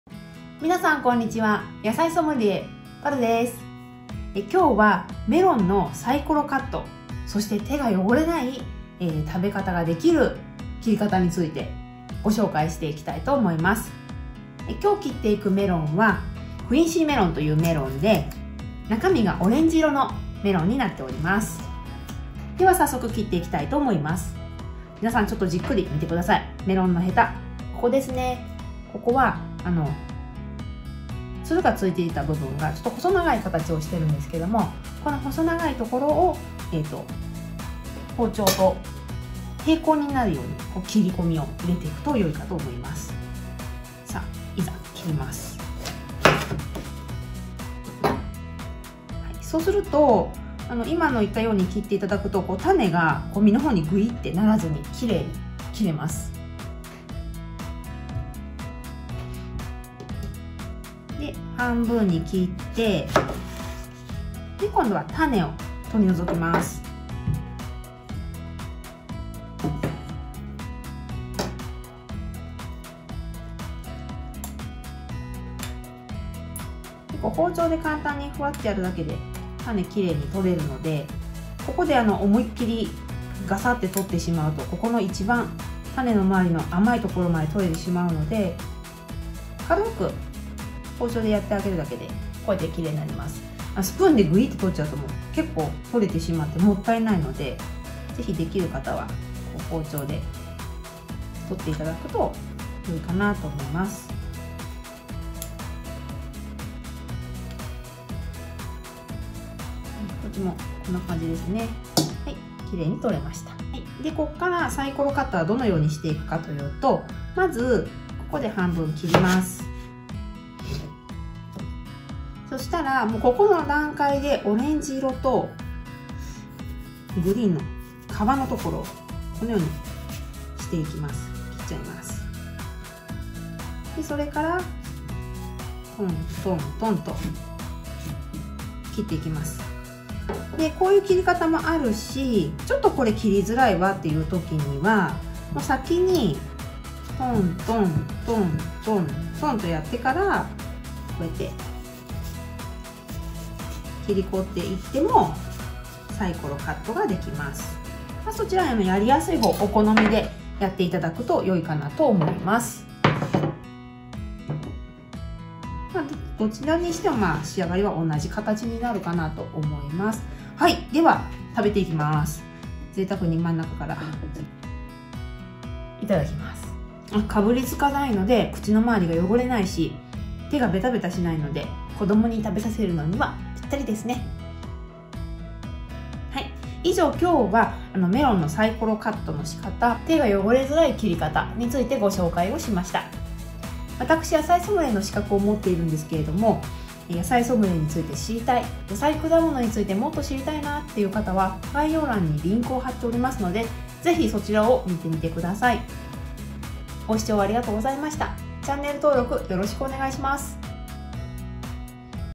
メロンの切り方！サイコロカットで食べるときに『手が汚れない！』
メロンのサイコロカットの切り方をご紹介！
メロンを食べる際に、手が汚れない切り方です。
ちょっとしたコツも紹介しています。

野菜ソムリエに興味のある方は、こちらから